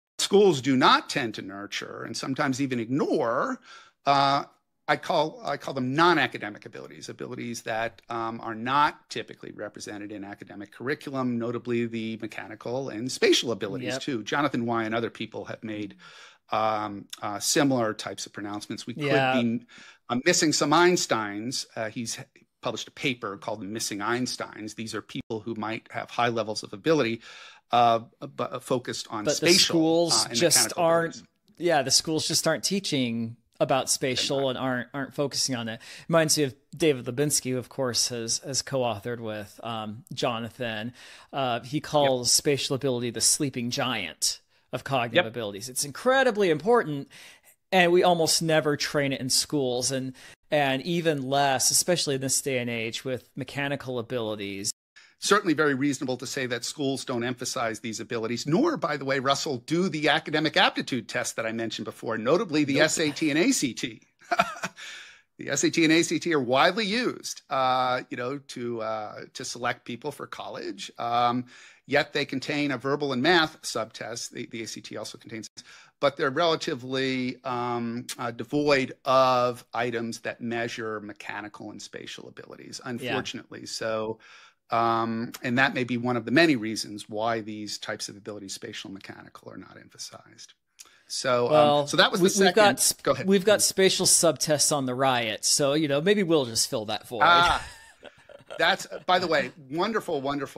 Are Schools Missing the Next Einstein
Schools and standardized tests like SAT/ACT ignore nonacademic abilities - particularly spatial intelligence - potentially overlooking future Einsteins, as researcher David Lubinski calls spatial ability the "sleeping giant" of cognitive abilities that education systems fail to nurture or measure. ⁣
➡️ Learn more and take a free sample of the RIOT IQ Test